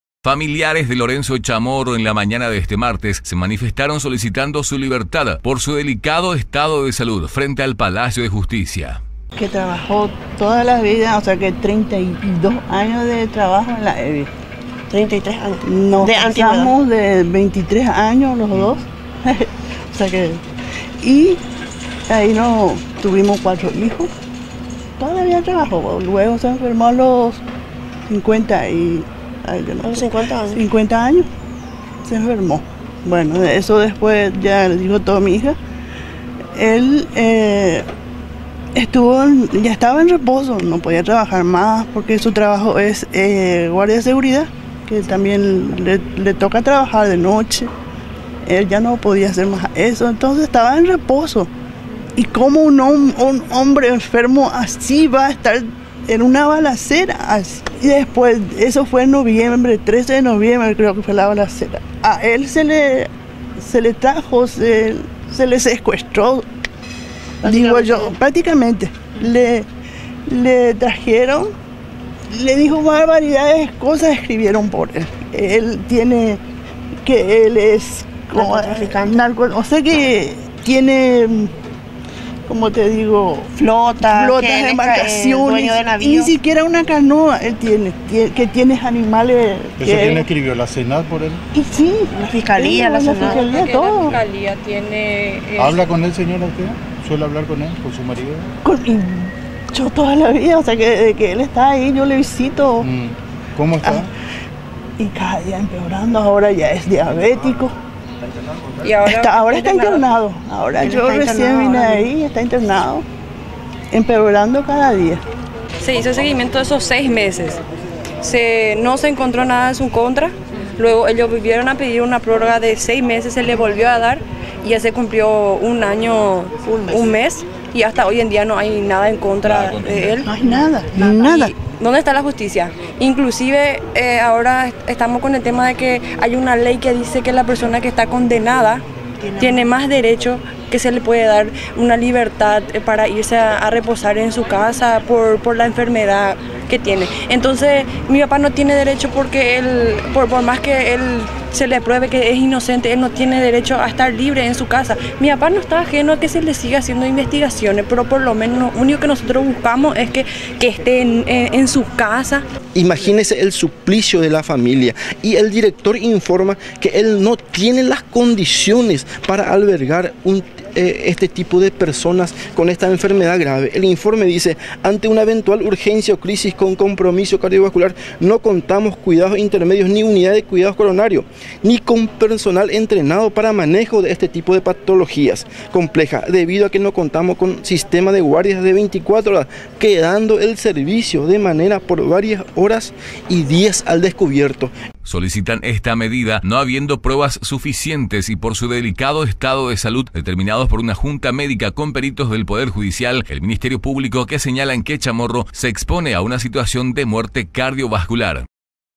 PIDEN LIBERTAD DE PROCESADO: LORENZO CHAMORRO PADECE DE UNA ENFERMEDAD GRAVE
Solicitan su libertad no habiendo pruebas suficientes en su contra delicado estado de salud en riesgo determinados por una junta médica con peritos poder judicial, del ministerio público que señalan que Chamorro se expone por su situación a una muerte cardiovascular,según la defensa en estos un año un mes del proceso no se ha encontró prueba suficiente para ligarlo al proceso más que las llamadas entre el mismo y hermanos González también detenido por la causa uno de ellos el capataz del establecimiento.

Otro de los aspectos reclamados por la defensa es que a pesar de su delicado estado de Salud no puede ser beneficiado con una medida que le permita estando imputado y procesado sin una condena no pueda tener el beneficio que ofrece el artículo 43 del código penal y 238 del código procesal penal que le permita el beneficio de la prisión domiciliaria, con el argumento de que aún no fue condenado.